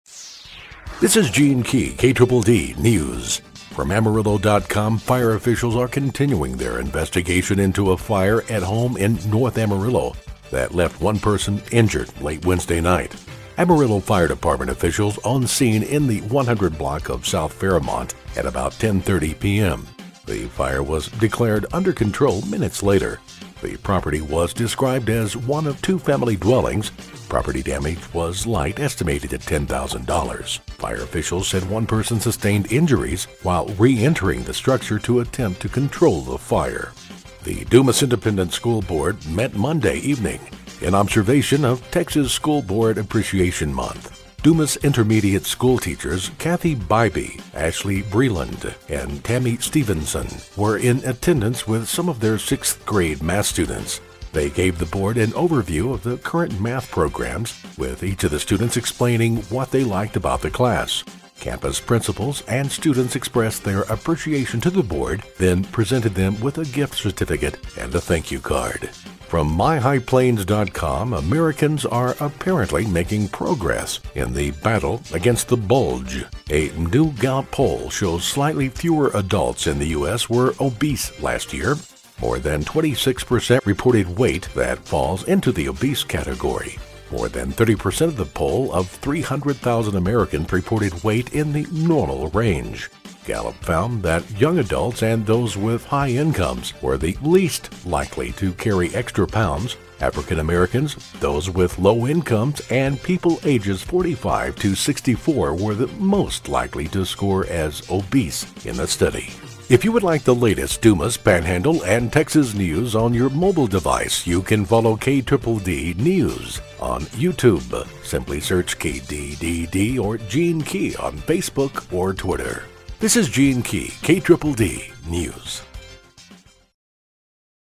KDDD Dumas News with Gene Key for 13 January Friday
You can follow KDDD on YouTube or Gene Key on Facebook or twitter.